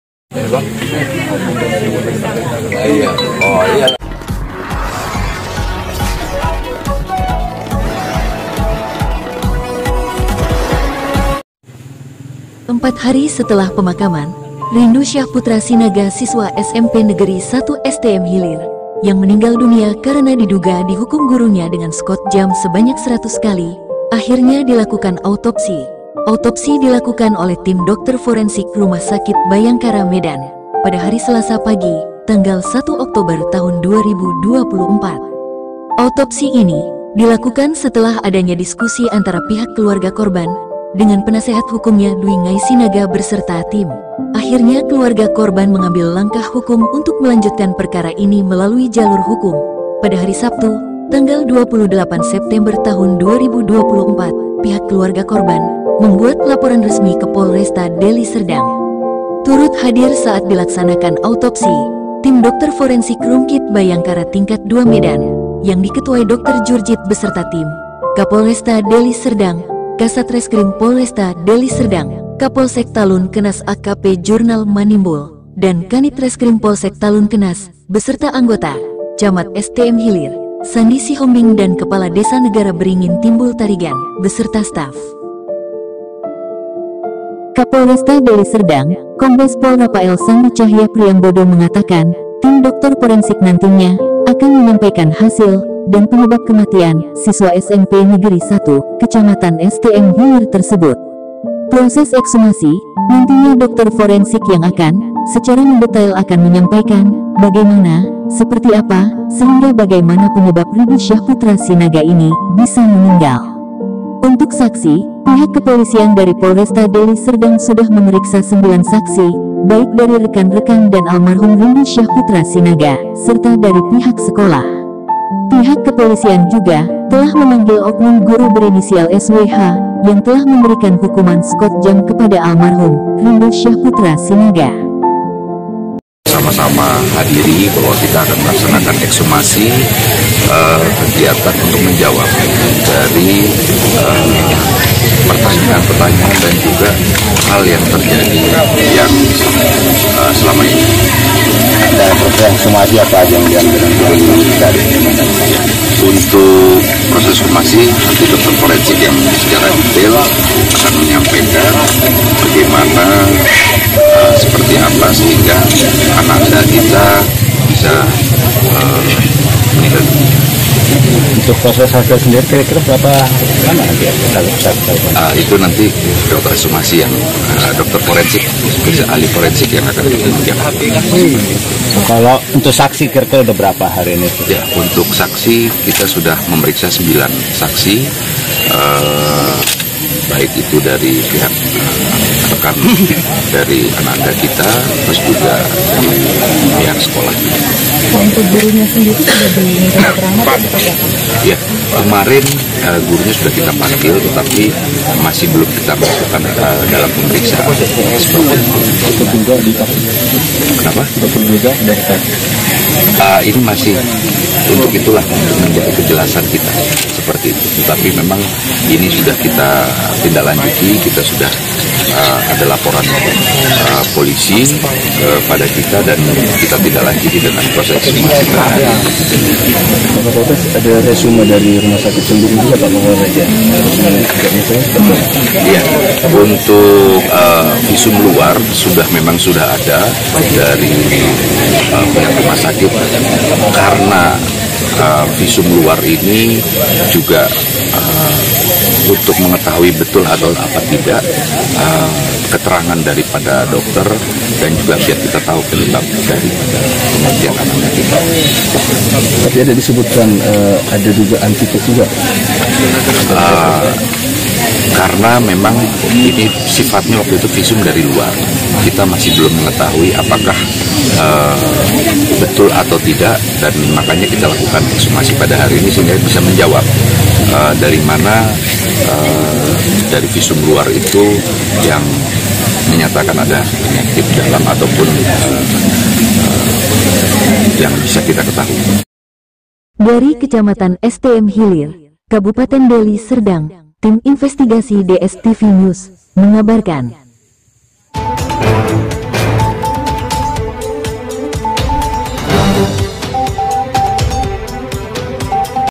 Tim Porensik Rumkit Bayangkara Medan Lakukan Ekshumasi dan Autopsi Usut Kematian Rindu Syahputra
Empat hari setelah pemakaman, Rindu Syahputra Sinaga (14) siswa SMP Negeri 1 STM Hilir yang meninggal karena dihukum gurunya dengan squat jump sebanyak 100 kali akhirnya dilakukan autopsi. Autopsi dilakukan oleh tim dokter forensik Rumkit Bhayangkara Tingkat II Medan. Pada selasa (01/10/2024) pagi.

Autopsi ini dilakukan setelah adanya diskusi antara pihak keluarga korban dengan penasehat hukumnya Dwi Ngai sinaga berserta tim. akhirnya keluarga korban mengambil langkah hukum untuk melanjutkan perkara ini melalui jalur hukum. Hari sabtu  (28/09/2024) pihak keluarga korban membuat laporan resmi ke Polres Deli serdang.

Turut hadir saat dilaksanakan autopsi tim dokter forensik Rumkit Bhayangkara Tingkat II Medan yang di ketuai dr Jurjit beserta tim, Kapolresta Deli Sedang Kasat Reskri
Kapolsek Talun Kenas AKP Jurnal Manimbul dan Kanit Reskrim Polsek Talun Kenas beserta anggota, Camat STM Hilir Sandi Sihombing dan Kepala Desa Negara Beringin Timbul Tarigan beserta staf.

Kapolresta deli serdang, kombes POL Rapael Sandy Chahya priambodo mengatakan, tim doktor porensik nantinya, akan menyampaikan hasil, dan penyebab kematian, siswa SMP negeri 1, kecamatan  STM hilir tersebut.

Proses ekshumasi, nantinya Dokter forensik yang akan, secara mendetail akan menyampaikan ,bagaimana, seperti apa, sehingga bagai mana penyebab Ridu Syahputra sinaga ini, bisa meninggal.

Untuk saksi, pihak kepolisian dari polresta Deli serdang sudah memeriksa sembilan saksi ,baik dari rekan rekan dan almarhum Rindu Syahputra Sinaga, serta dari pihak sekolah.

Pihak kepolisian dari polresta Deli serdang juga, telah memanggil oknum guru berinisial SWH, yang telah memberikan hukuman skot jam kepada almarhum, rindu Syahputra sinaga